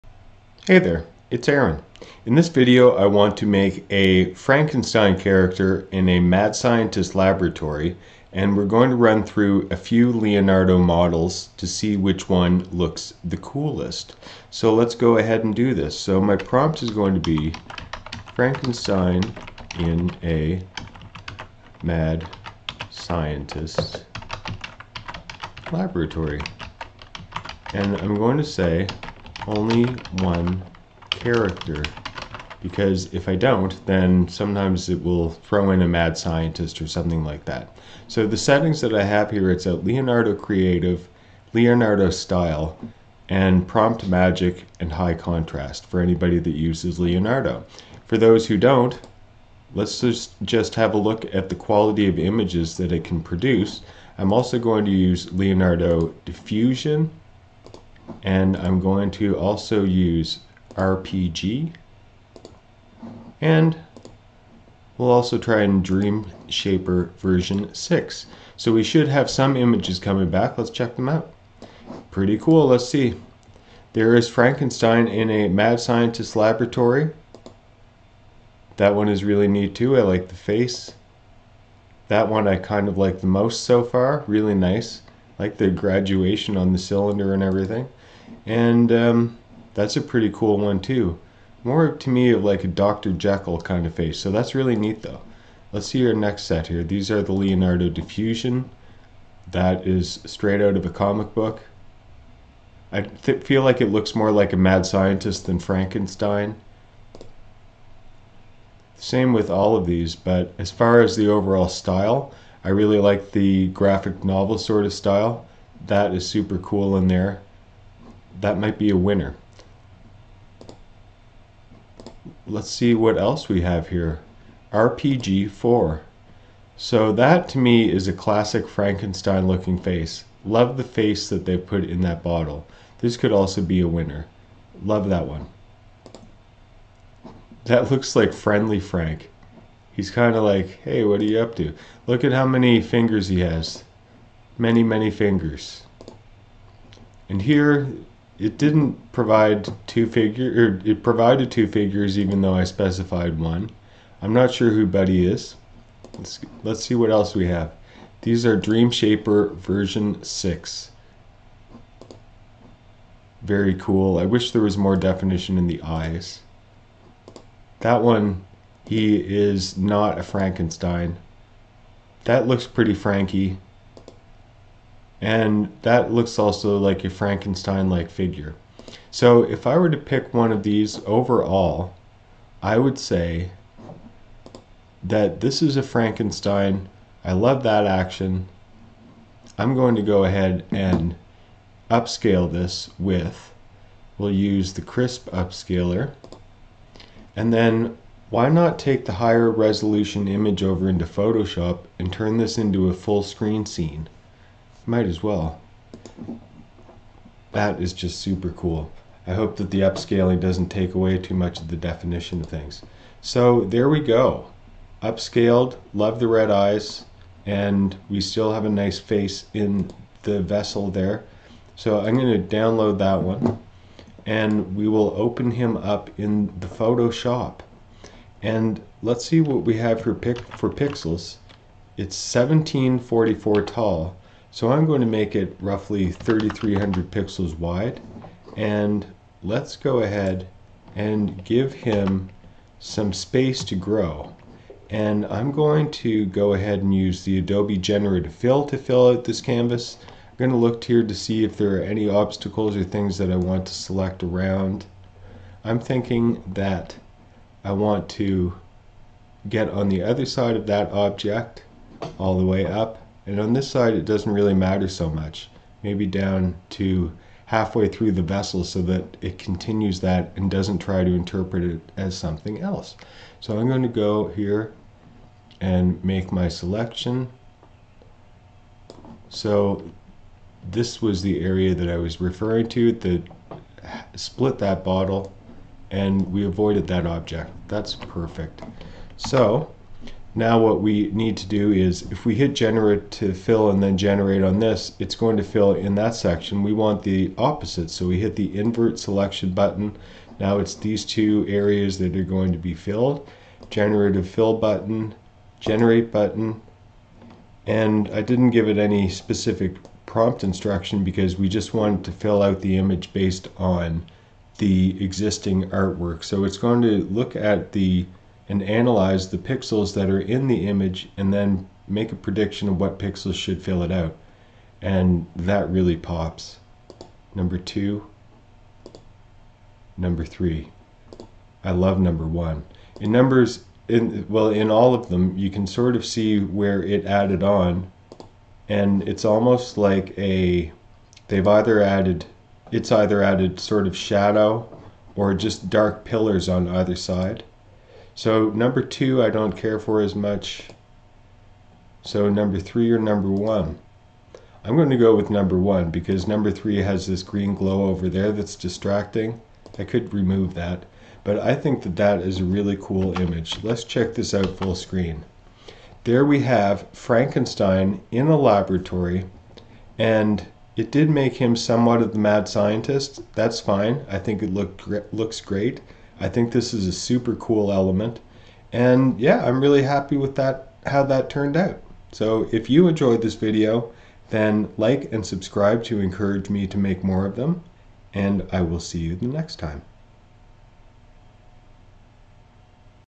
Creating Frankenstein with AI - Leonardo.ai and Photoshop AI Generative Fill
In this YouTube video, Aaron explores different Leonardo models to create a Frankenstein character in a mad scientist laboratory. He uses various settings and prompts to generate images and selects his favorite. He then upscales the image and uses Photoshop's AI generative fill to expand the canvas and create a full-screen scene.

Highlights
👍 Aaron tests different Leonardo models to create a Frankenstein character in a mad scientist laboratory.
🖼️ He selects his favorite image with a cool graphic novel style and upscales it.
🎨 Using Photoshop's AI generative fill, he expands the canvas and creates a full-screen scene, achieving an impressive result.